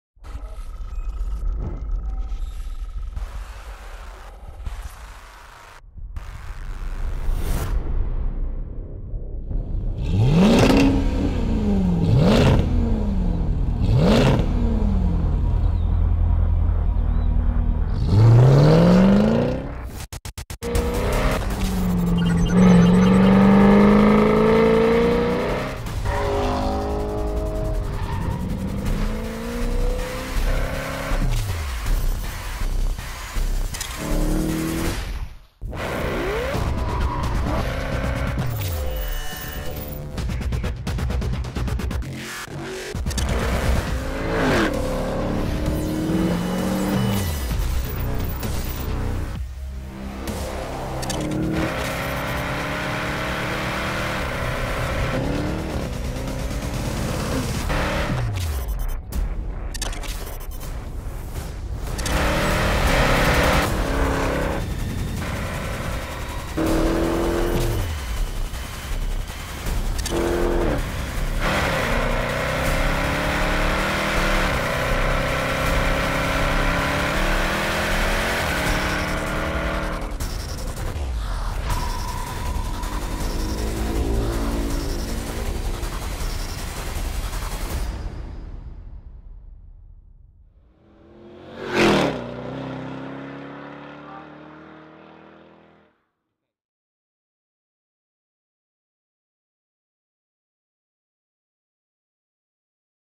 Jaguar F-Type SVR @ racetrack
Jaguar F-Type SVR
Motore 5,0 litri V8 sovralimentato
575 cv e 700 nm di coppia
da 0 a 100 km/h in 3,7 secondi
velocità massima di 322 km/h